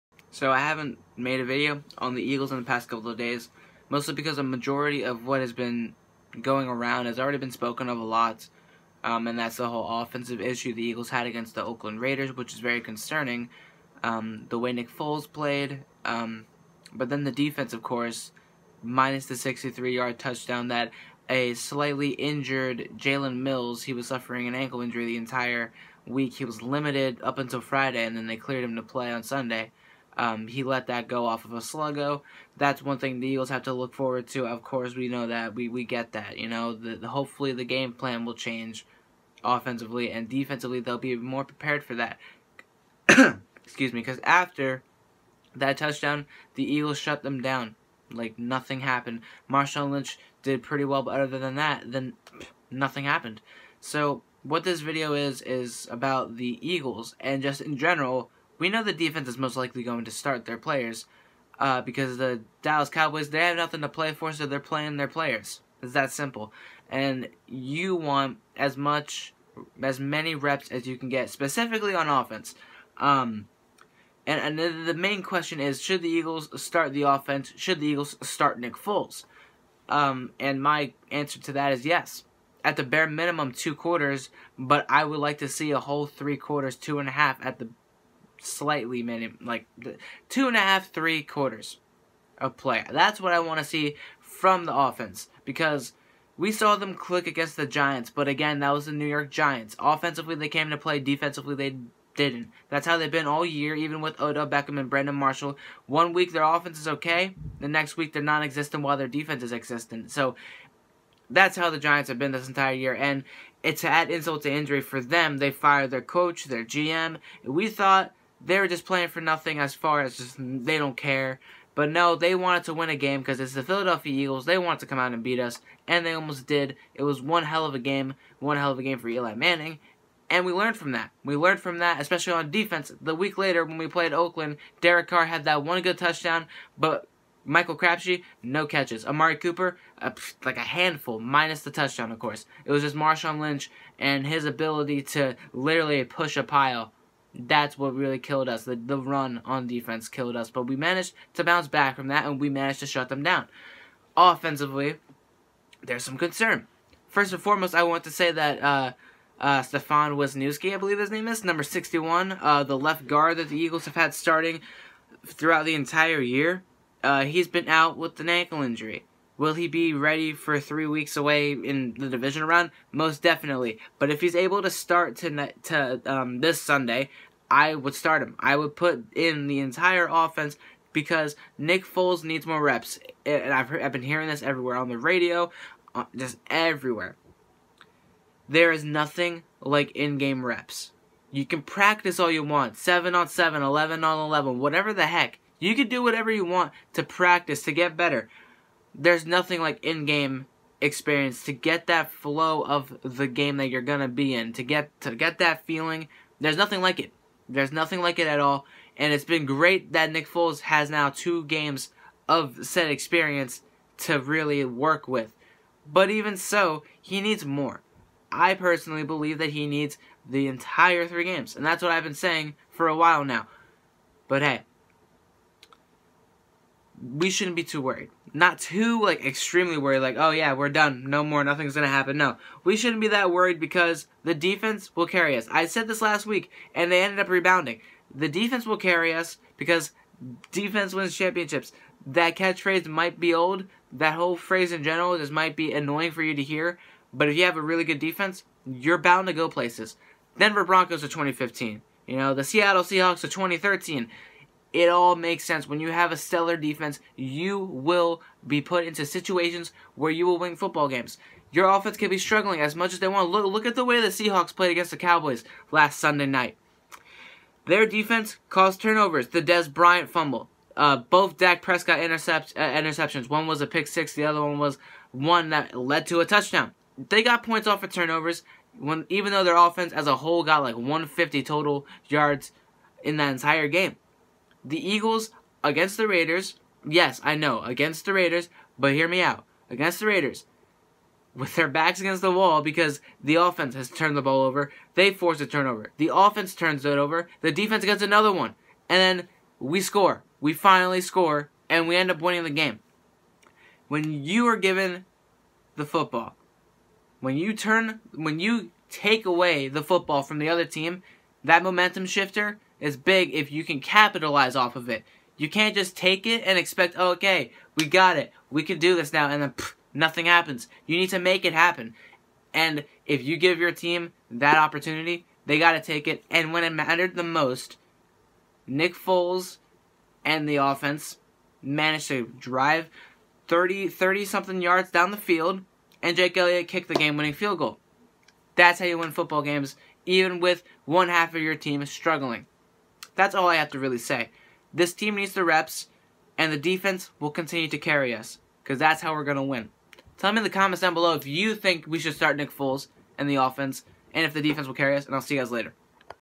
Eagles Must Start Foles vs Cowboys
My thoughts after the Oakland game.